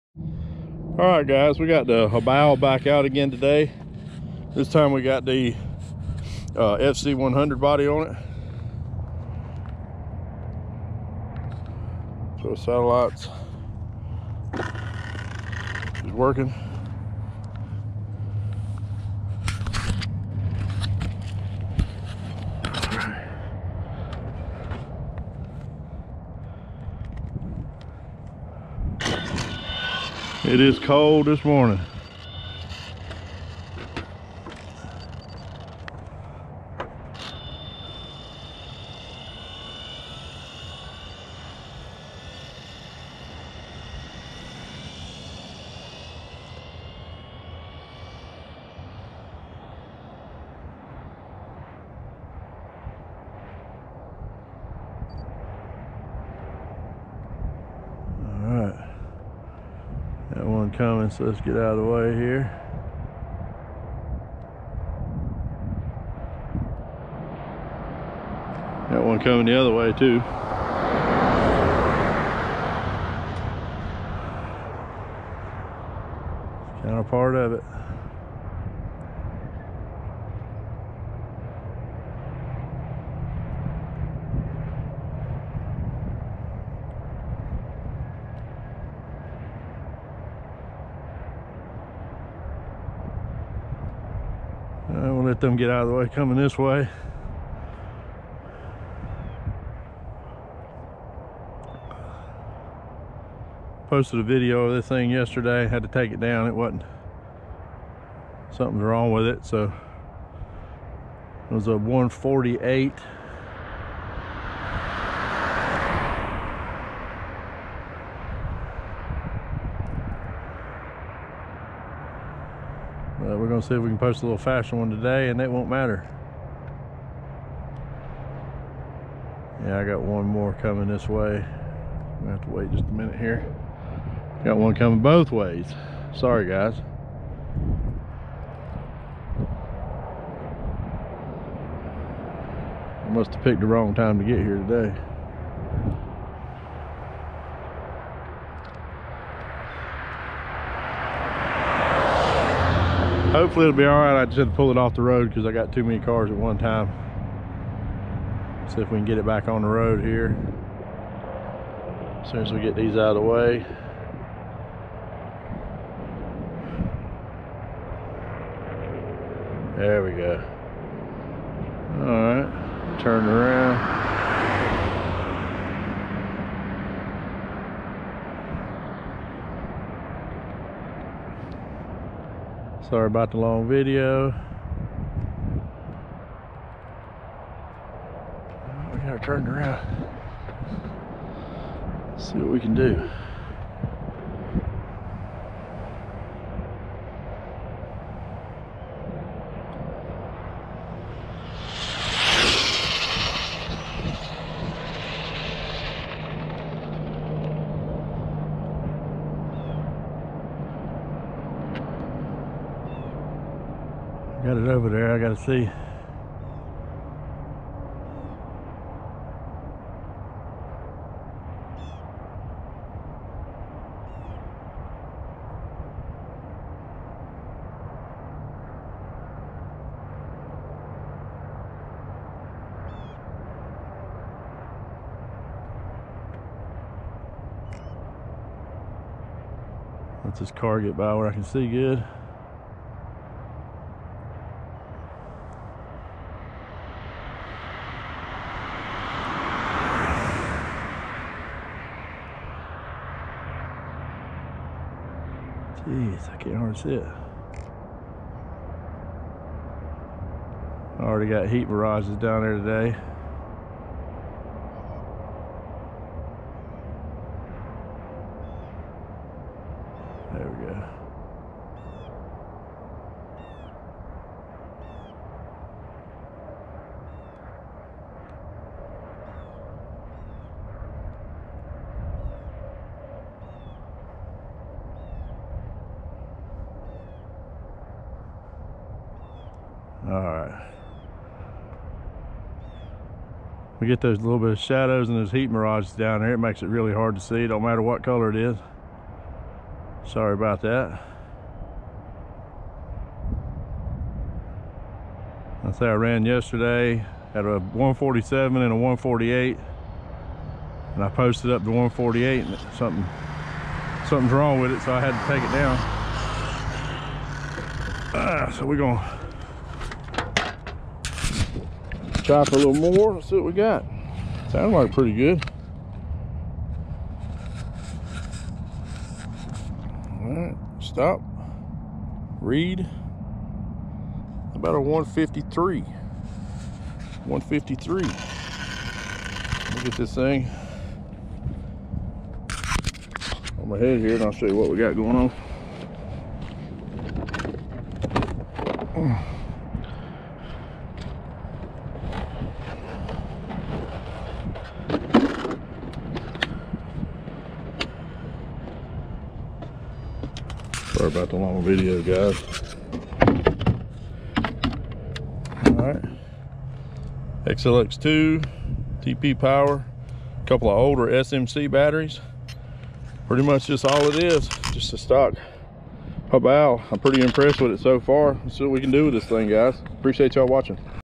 HOBAO VTE2 SPEED RUN , getting faster. NEW PB 153mph.
Going to push this Hobao build and see what it can do,  impressive so far